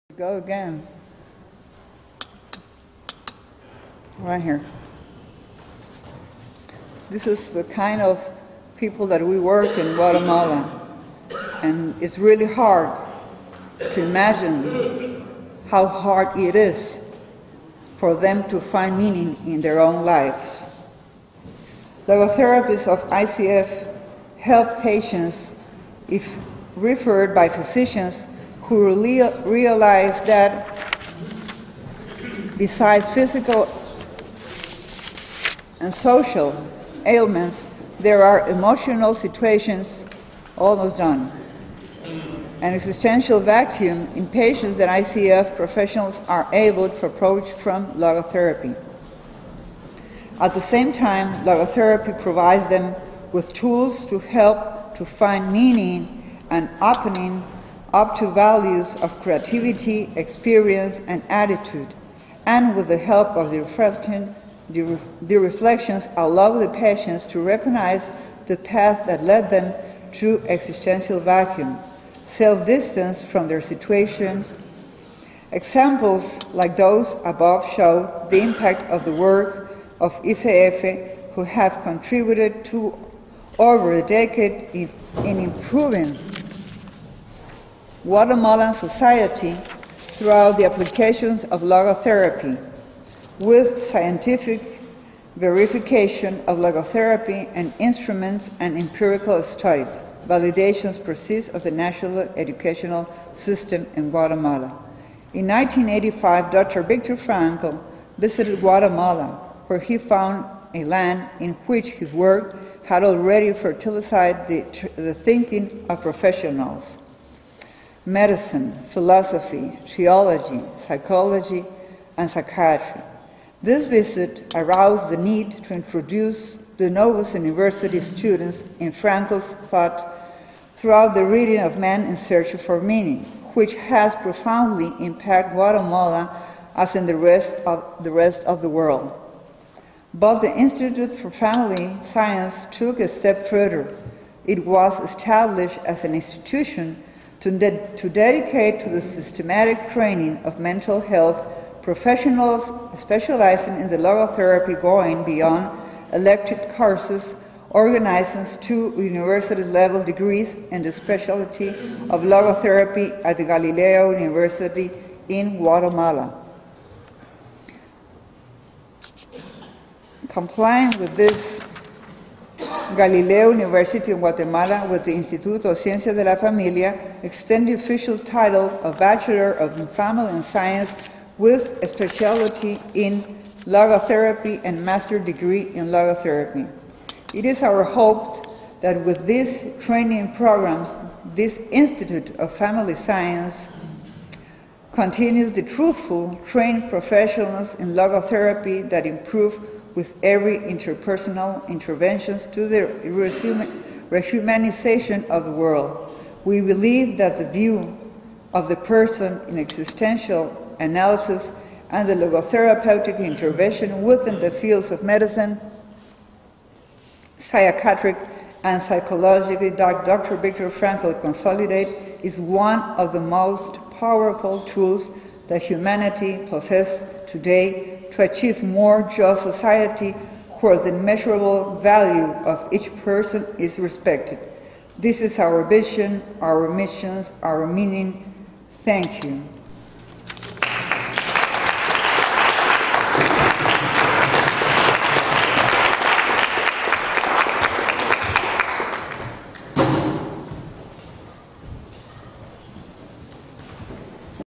Ma. Ángeles Mollinedo - The Guatemala Experience at the Institute of Family Sciences (ICF)
MOLLINEDO, MARIA DE LOS ÁNGELES [MA. MA.] [Guatemala]
With the important innovative advances of the mental health studies in the neurosciences, and the demanding need for scientific verification and statistical measure of research work, the mental health professionals are more inclined towards scientific approaches in the fields of psychology and psychiatry. In this scientific school of thought, Logotherapy is often viewed and reduced to a humanistic approach in the trend of positive psychology, or as part of the self-help movement...